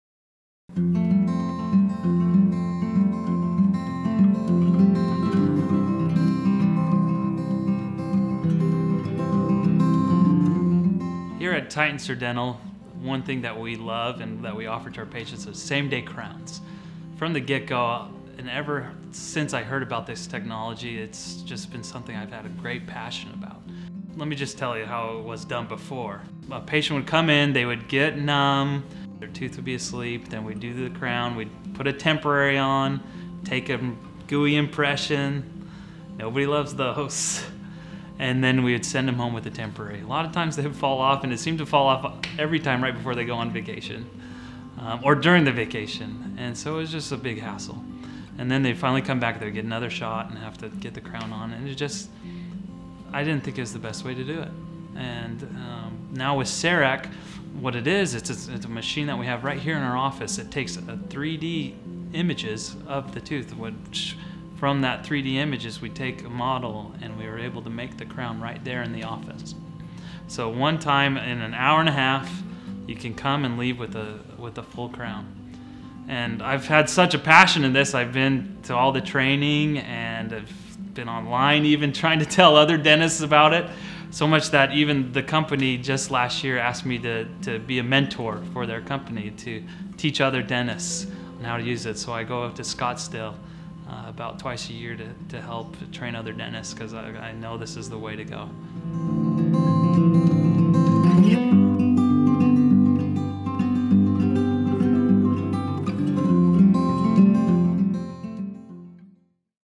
Cerec Same Day Crowns at Titensor Dental
At Titensor Dental, they offer CEREC same-day crowns to their patients. They recognize their patients have busy schedules and the difficulty they may have in scheduling multiple dental visits. When their recommendation for dental treatment includes the restorative treatment of a dental crown, they can use the CEREC machine to offer same-day dental crowns. Please note, faster production does not mean lesser quality. By using superior 3-D photography and CAD/CAM technology, Dr. Titensor programs the CEREC machine to make dental crowns that are durable and properly contoured to fit your tooth.

Titensor Dental
1901 Long Prairie Rd., Suite 320
Flower Mound, Texas 75022